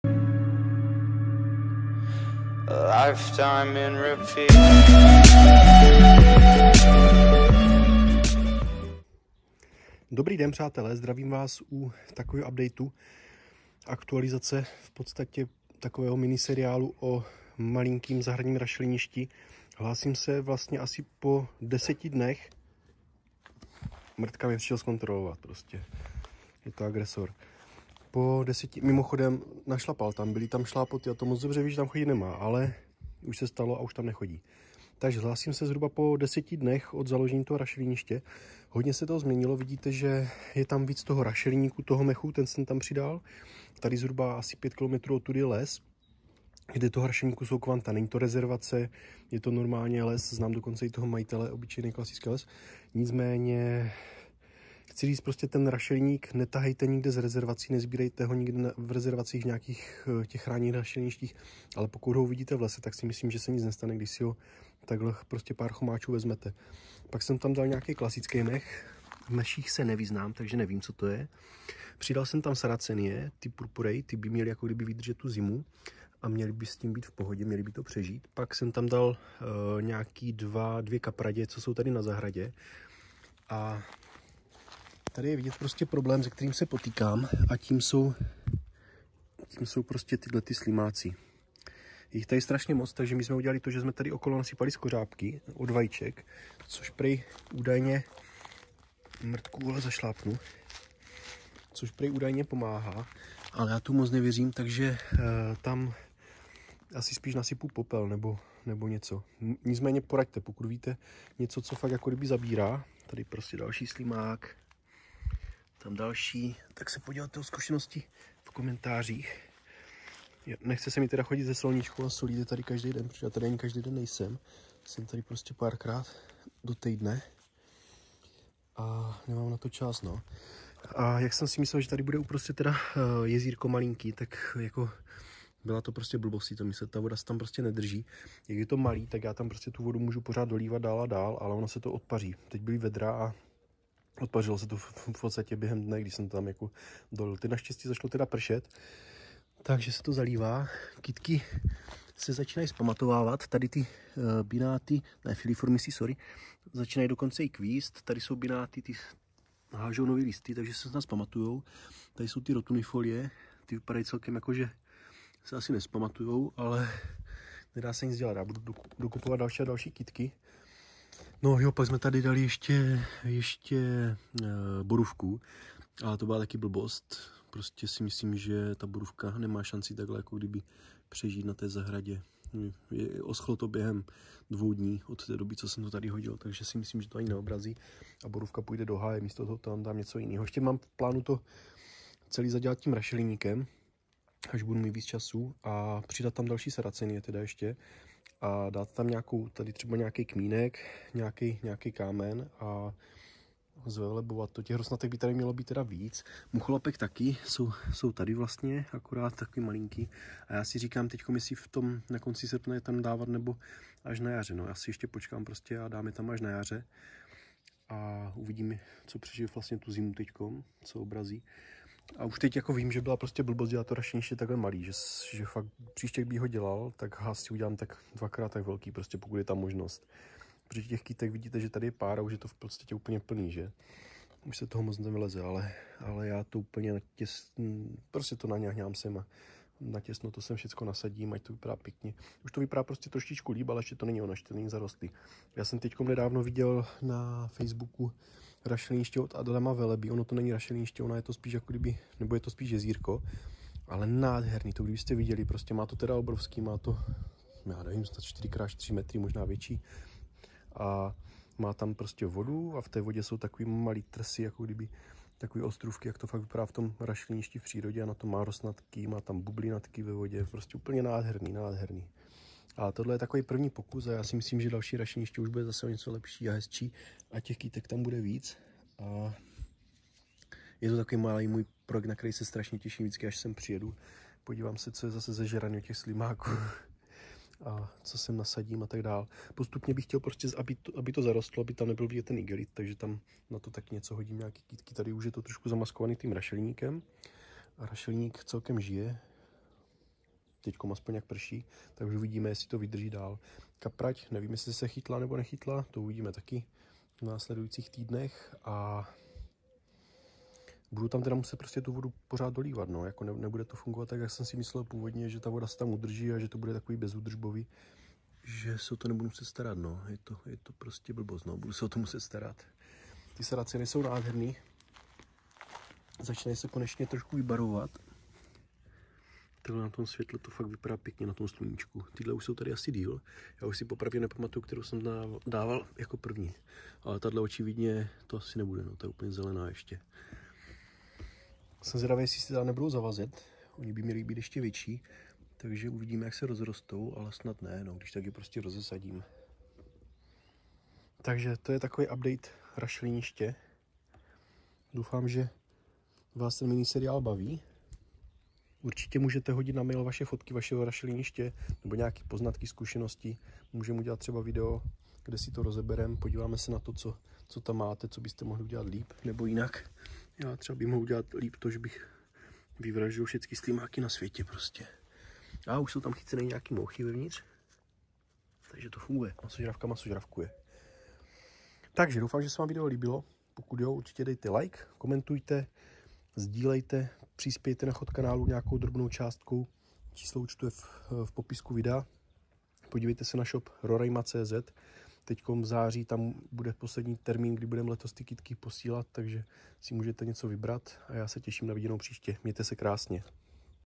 Jak si postavit zahradní rašeliniště? Díl 3
Další pokračování seriálu o zahradním rašeliništi 😃

Přispět na chod kanálu můžete na účet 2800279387 / 2010

Odkazy na potřeby pro masožravkáře

Sociální sítě

Použitá technika